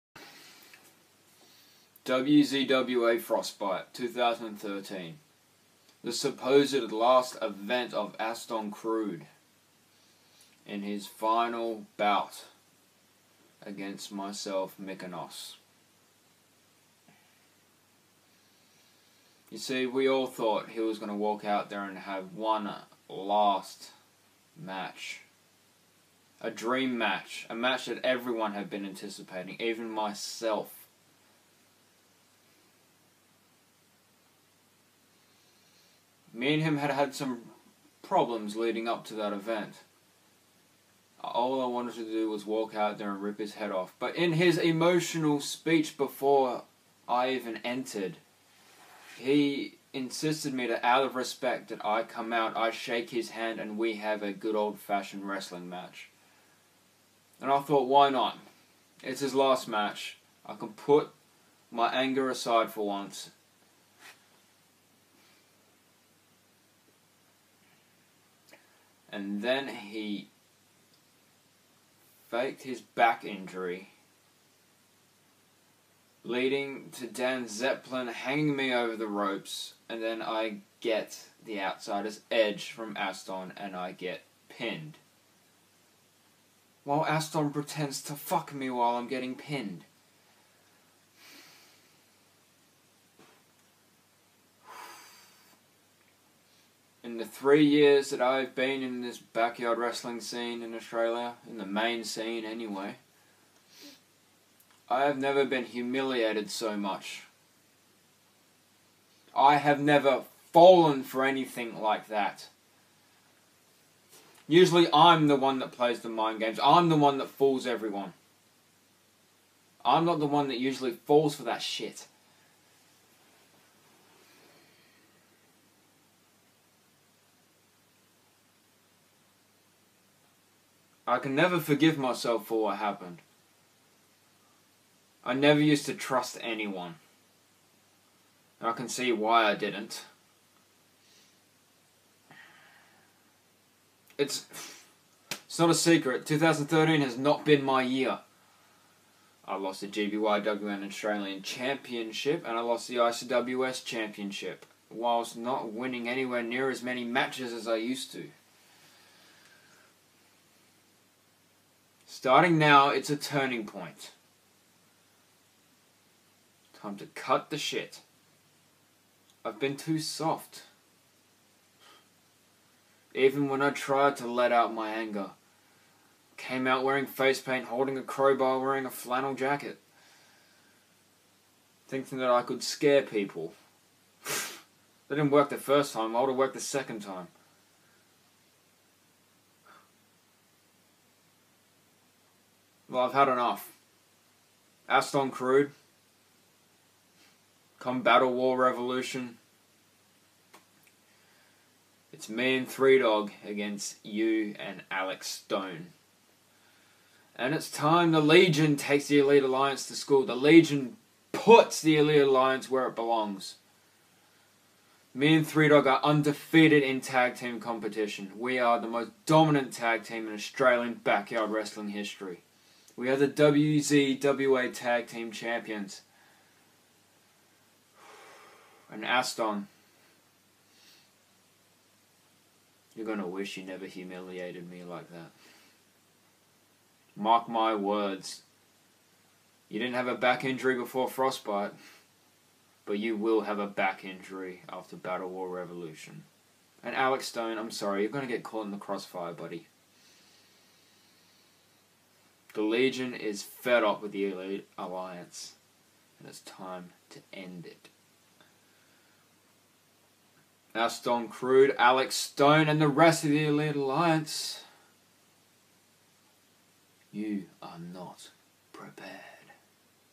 WZWA BattleWar Revolution 2013 - Mikanos speaks on Aston Crude
After being embarrassed and beaten by Aston Crude and the Elite Alliance at Frostbite, Mikanos took a month off to gather his thoughts on the situation. Now at BattleWar Revolution he and his tag partner 3-Dawg take on Crude and a partner of his choosing for the WZWA Tag Team Championship.